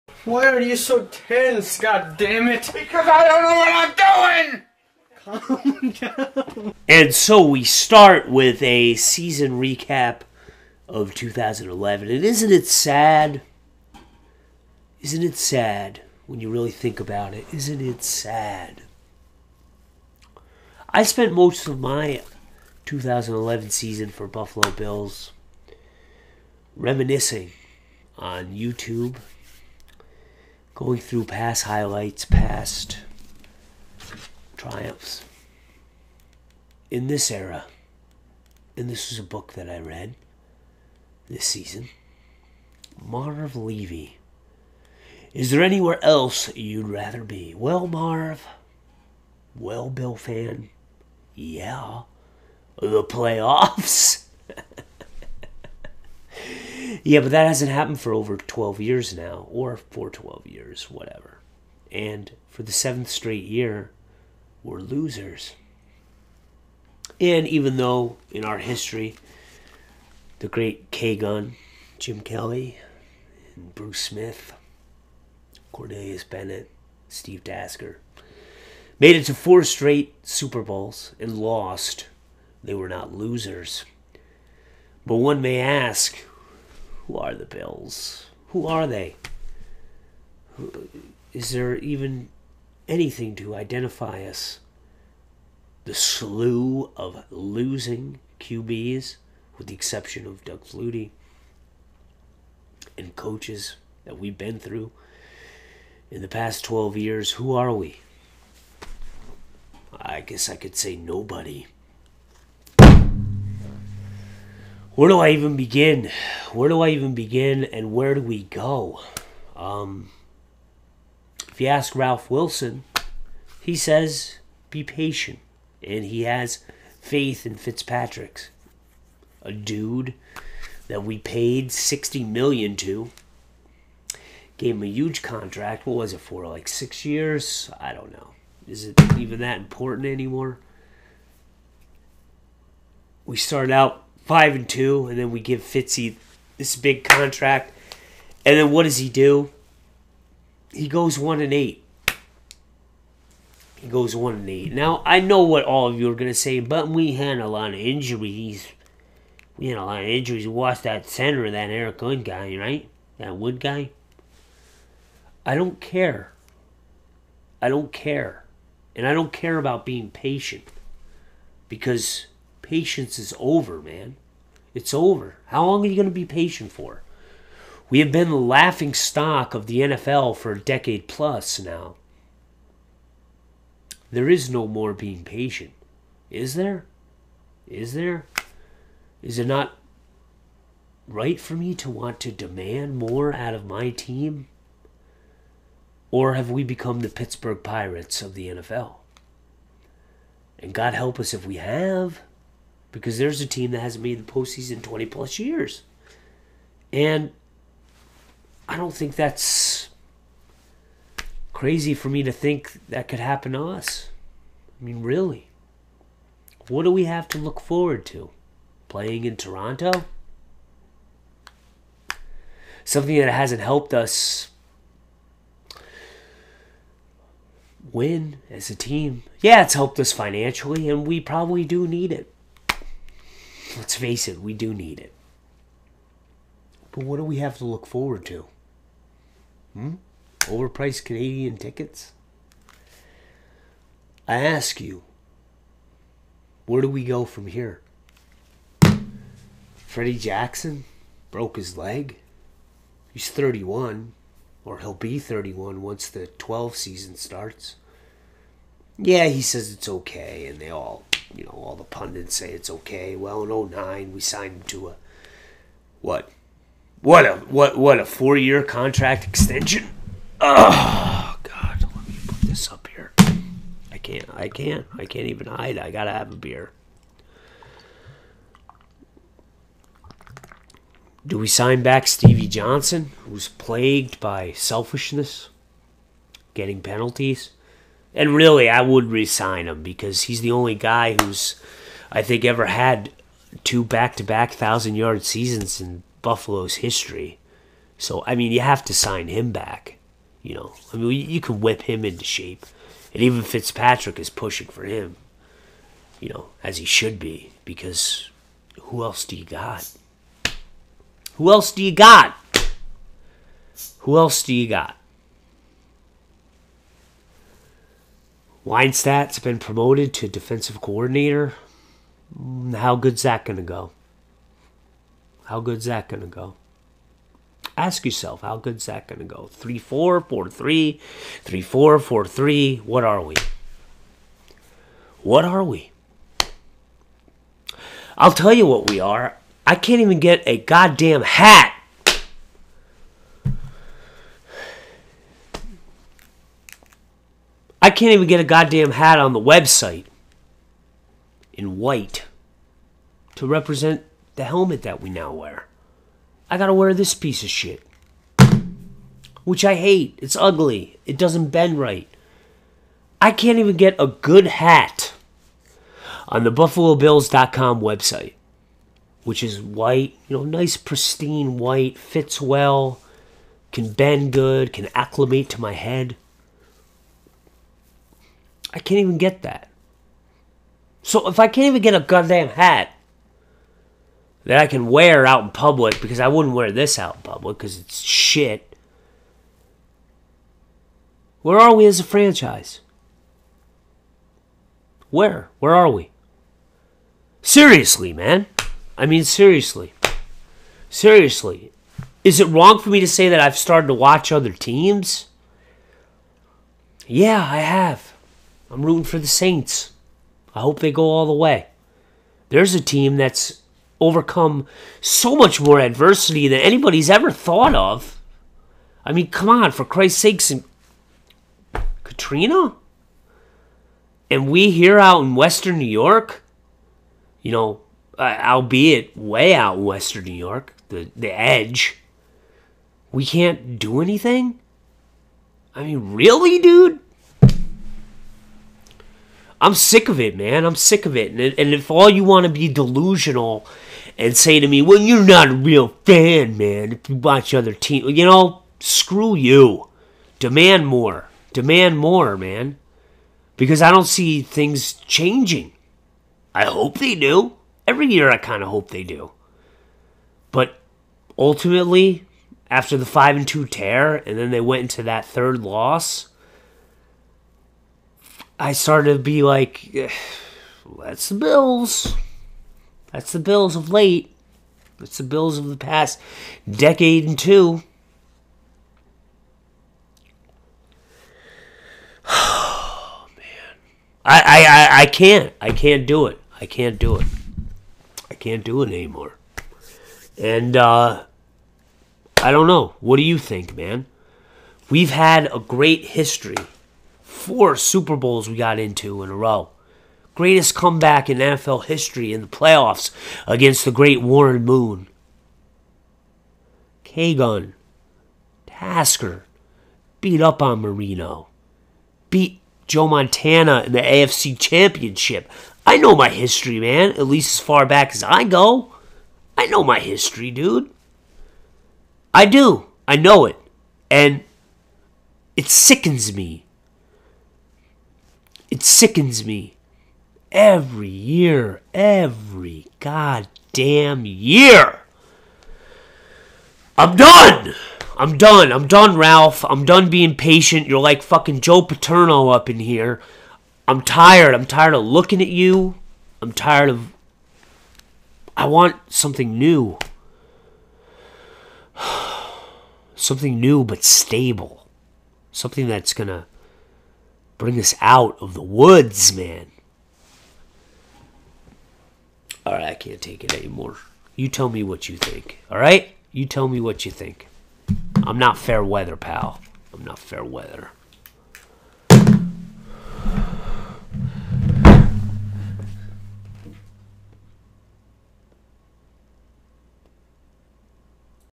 Buffalo Bills 2011 Season Review We Suck Recap
I'm tired.  As a bills fan i can't wait anymore.  Twelve years and no playoffs is long enough.   Forget about it, what the hell else can I do.  I can't even get a decent hat on the website--- I'm done... It's over .  Do Bills fan even own computers?  Why the internet silence on the magnitude of our sucKitude?  Where's the outcry?
~Kuts~